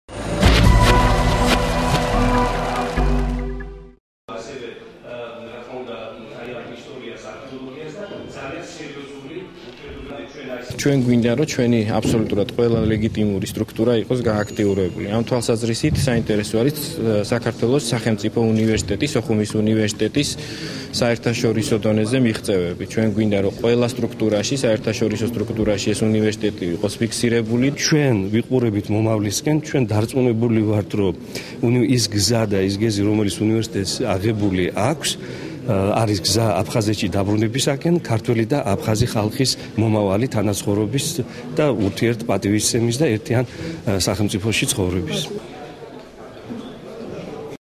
23.12.09
ტერიტორიული მთლიანობის აღდგენის საკითხთა დროებით კომისიაზე სოხუმის სახელმწიფო უნივერსიტეტის რექტორს უმასპინძლეს